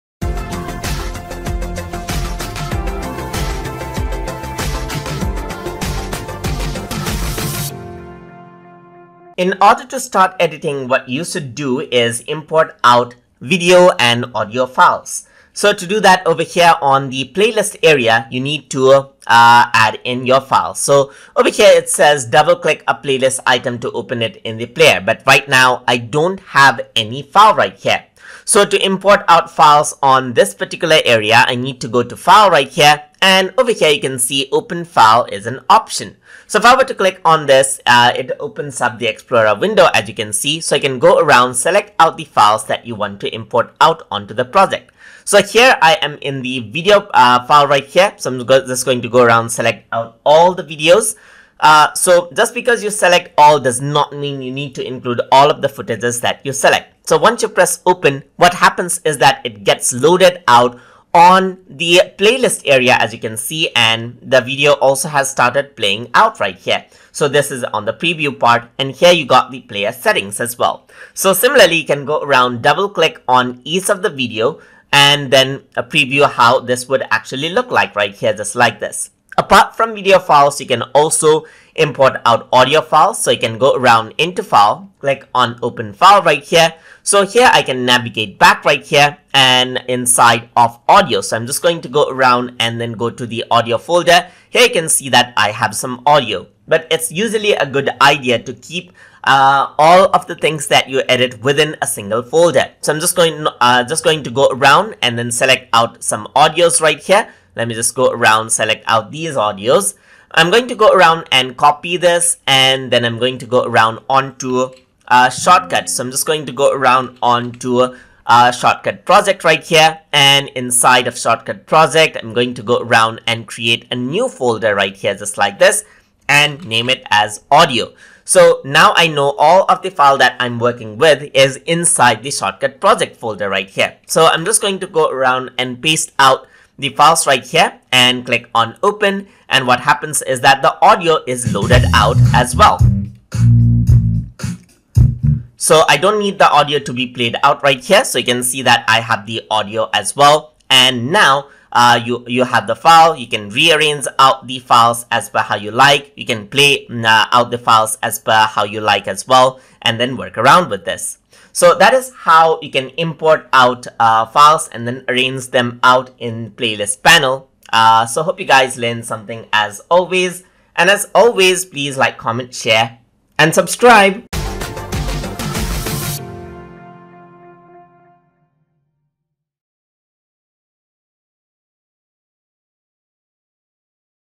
Shotcut Tutorial - Lesson 4 - Importing Files and arranging them in Playlist Panel
In this tutorial, we will be discussing about Importing Files and arranging them in Playlist Panel in Shotcut

Learn the basics of using Shotcut including how to cut, edit, manage and export videos.

This lesson has been made using Shotcut version 22 but is applicable for older and newer versions of the application as well